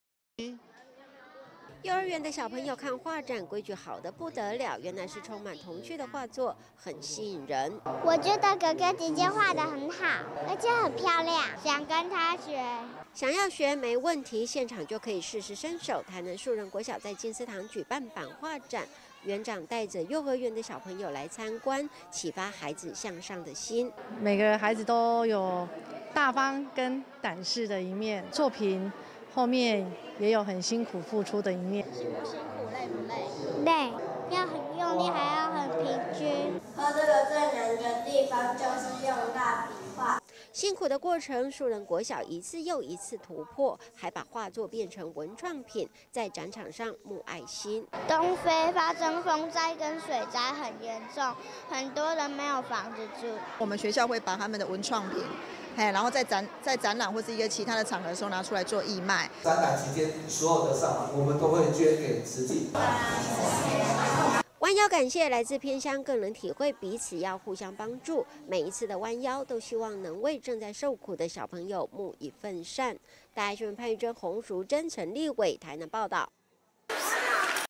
樹人國小 送愛東非
47個國家地區的慈濟志工，持續募心募愛。有敘利亞難民或是貧民居的居民，都有錢出錢有力出力，而在台灣，學生也是愛心不落人後，台南樹人國小，版畫在台南展出，學生把畫作的文創品拿來募心募愛，全校師生更決定，展期內文創品所得通通捐出來給慈濟的國際賑災。

幼兒園的小朋友看畫展，規矩好得不了，原來是充滿童趣的畫作，很吸引人。

台南育仁幼兒園學生 黃楚喬：「我覺得哥哥姊姊畫得很好，而且很漂亮。」

台南育仁幼兒園學生 吳羽晨：「想跟他學。」

想要學沒問題，現場就可以試試身手，台南樹人國小在靜思堂舉辦版畫展，園長帶幼兒園的小朋友來參觀，啟發孩子向上的心。

台南育仁幼兒園園長 許惠淨：「每個孩子都有大方跟膽識的一面，作品後面也有很辛苦付出的一面。」

台南育仁幼兒園學生 陳婕羽：「(辛不辛苦 累不累)，累 要很用力 還要很平均。」

辛苦的過程，樹人國小的孩子一次又一次突破，還把畫作變成文創品，在展場上募愛心。

台南樹人國小學生 洪佳瑩：「東非發生風災跟水災很嚴重，很多人沒有房子住。」

台南樹人國小老師 洪千婷：「我們學校會把他們的文創品，在展覽或其他的場合，拿出來做義賣。」

台南樹人國小校長 劉裕承：「展覽期間所有的善款，我們都會捐給慈濟。」

彎腰感謝，來自偏鄉更能體會彼此要互相幫助，每一次的彎腰，都希望能為正在受苦的小朋友募一分善。

潘俞臻 洪淑真 陳立偉 台南報導